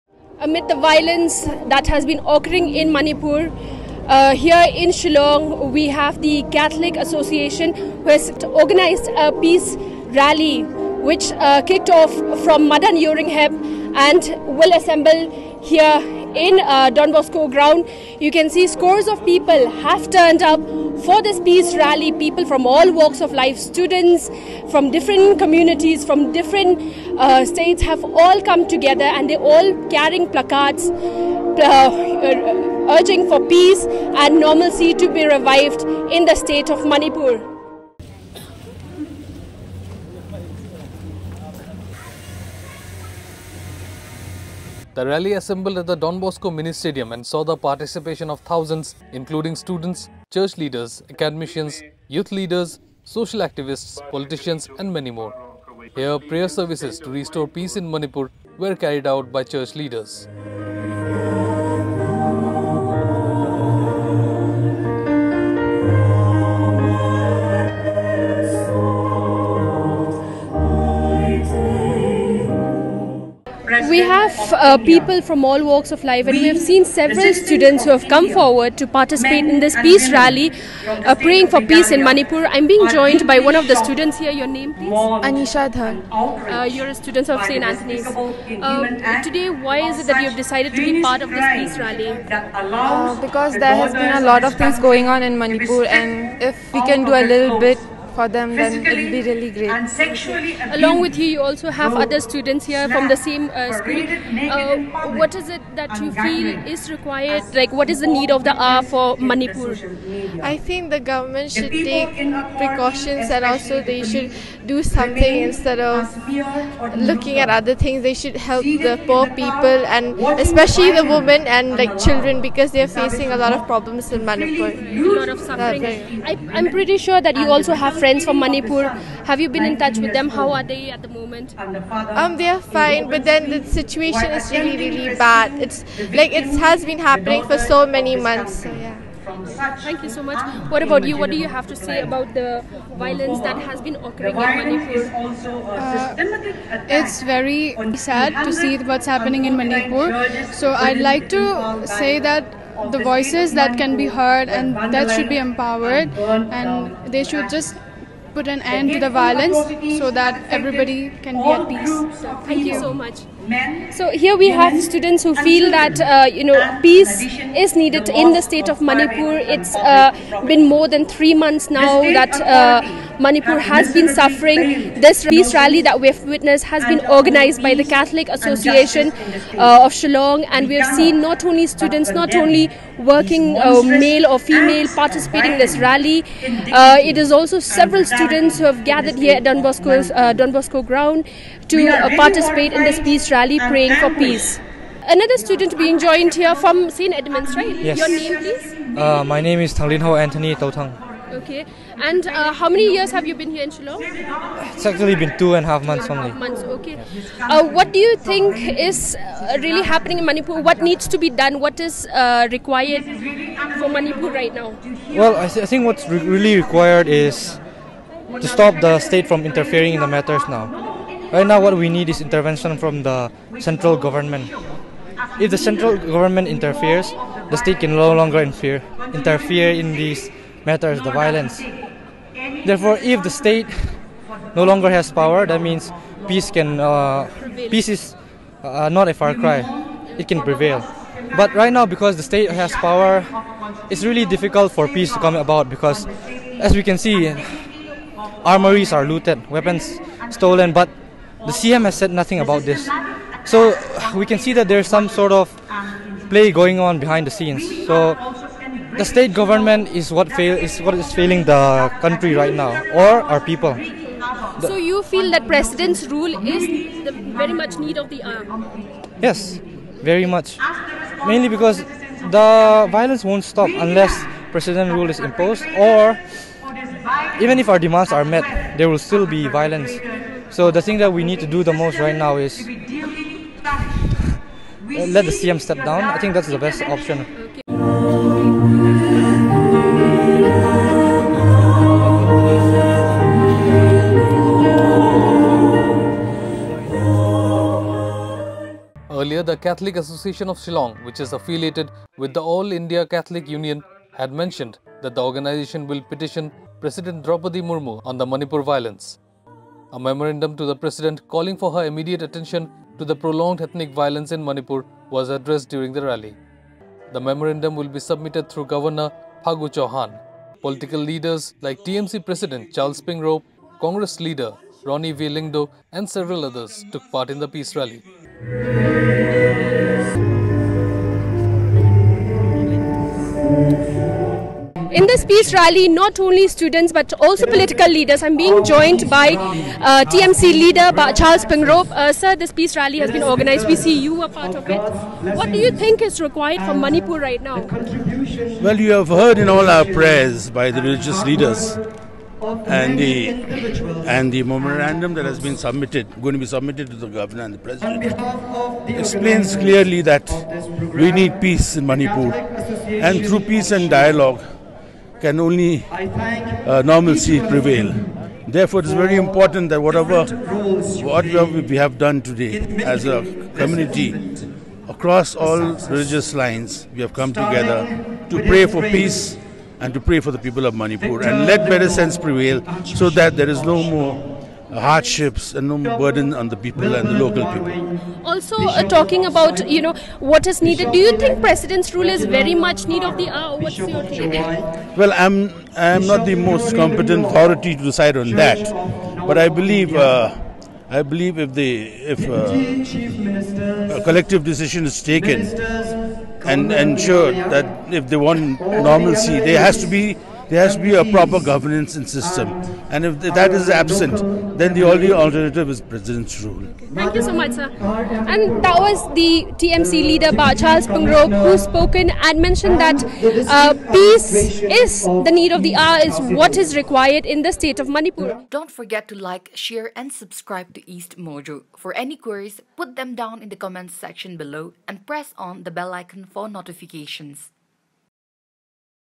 Thousands take to Shillong streets after church body calls to end ethnic violence in Manipur
The rally start from Madan Iewrynghep at 10 am and proceeded through the Laitumkhrah area towards Don Bosco Square and assembled at the Don Bosco Mini Stadium to offer prayers for peace in Manipur.

The rally saw the participation of thousands including students, church leaders, academicians, youth leaders, social activists, politicians and many more. Political leaders like health minister, TMC president Charles Pyngrope, Congress leader Ronnie V Lyngdoh and several others took part in the peace rally.

At the mini stadium, prayers services to restore peace in Manipur were carried out by church leaders.

A memorandum to the President calling for her immediate attention to the prolonged ethnic violence in Manipur was addressed during the rally. The memorandum will be submitted through Governor Phagu Chauhan.